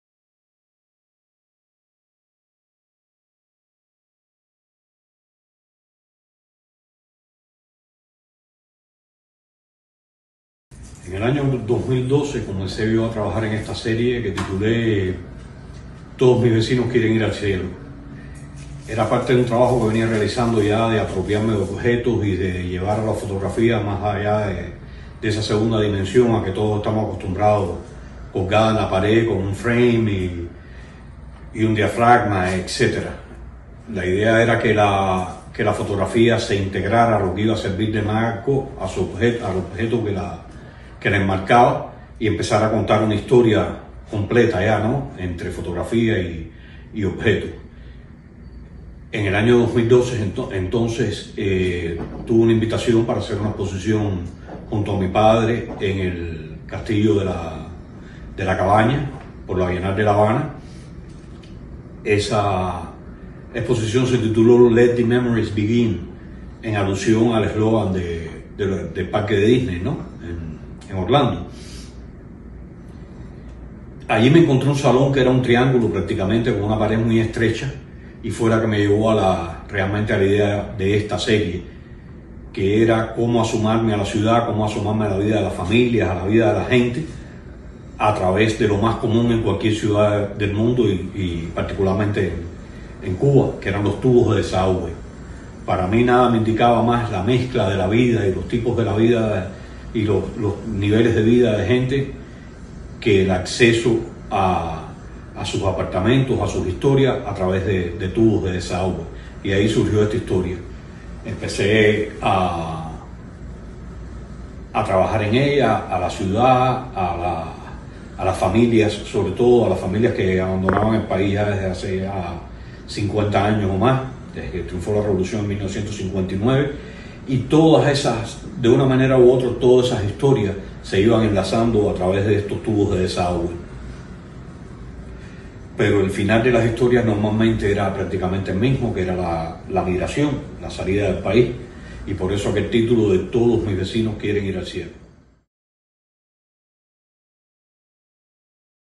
Ernesto Javier Fernández in the exhibition "Under the Spell of the Palm Tree"
🗣️ Artist's Conversations

To the artists participating in the exhibition "Under the Spell of the Palm Tree: The Rice Collection of Cuban Art" at the Harn Museum of Art, the museum asked the following question:

What would you like audiences to take away from the experience of viewing your work in the exhibition?

In this video you can watch artist Ernesto Javier Fernández answering the question.

Exhibition curated by Gabriela Azcuy and David Horta